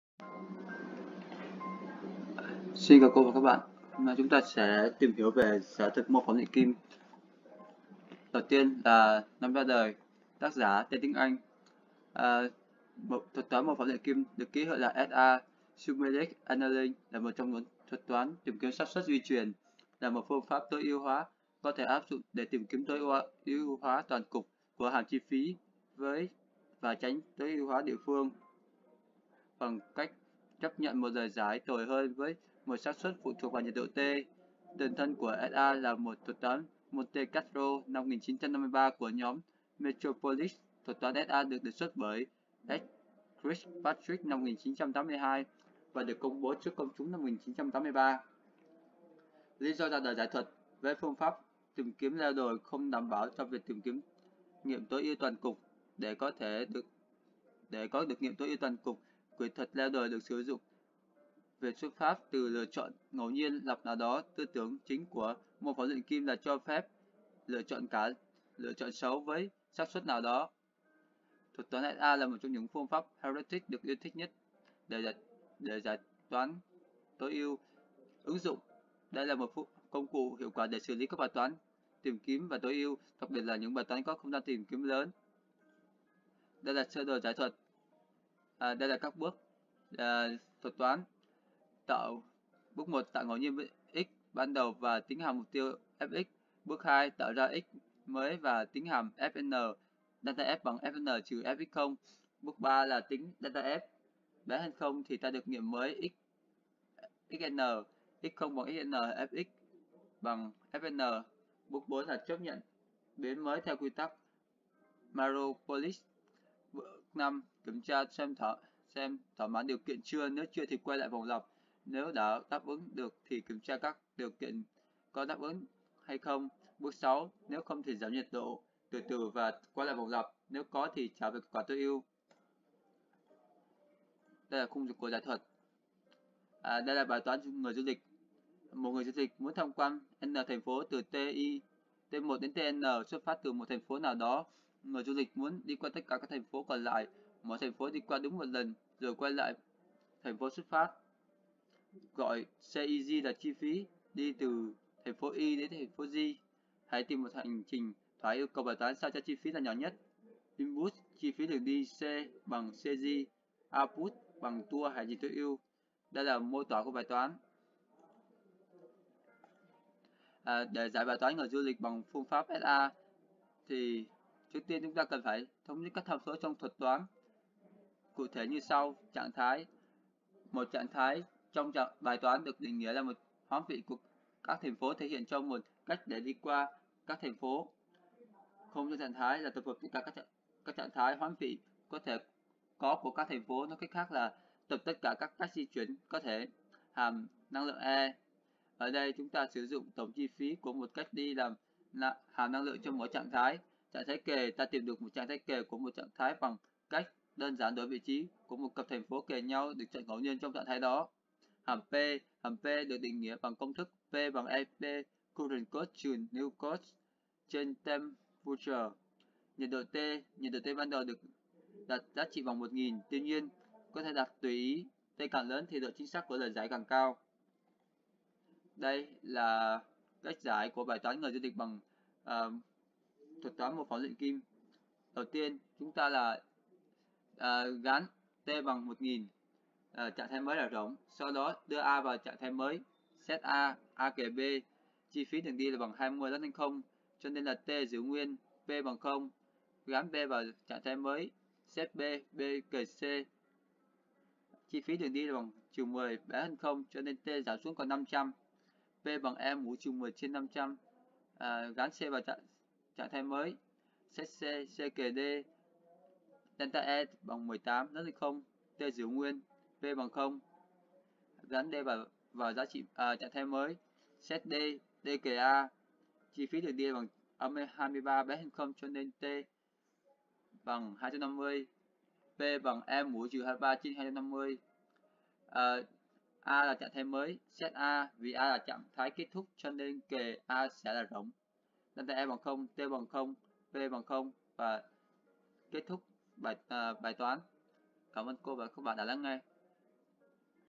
Tổng quan và ví dụ chi tiết giải thuật Mô Phỏng Luyện Kim - Tlu
AI in Education
Trí tuệ nhân tạo